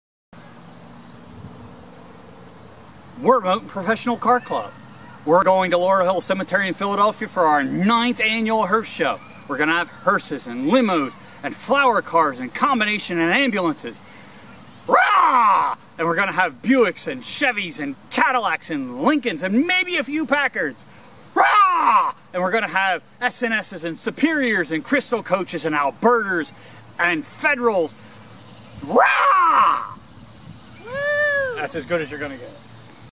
The 9th MPCC ProCar Show at Laurel Hill Cemetery
Teaser for the 9th MPCC ProCar show at Laurel Hill Cemetery (Philadelphia), on August 12th from 10-4.